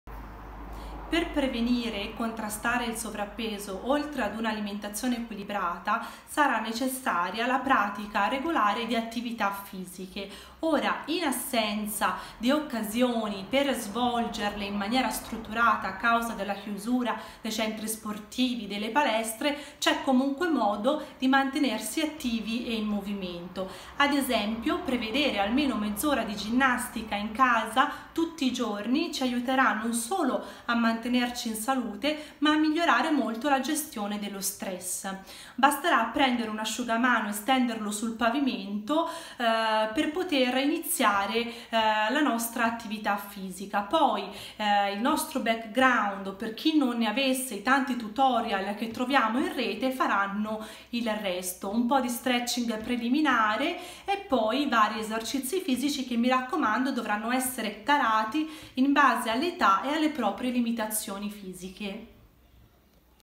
Nutrizione, stile di vita e attività fisica ai tempi del Coronavirus: part 5
La dott.ssa Benedetta Paoletti dietista presso il Centro Mindfulness Firenze ci dà preziosi consigli per alimentarci con consapevolezza, scegliendo alimenti che favoriscono il benessere, l'energia e il mantenimento della linea.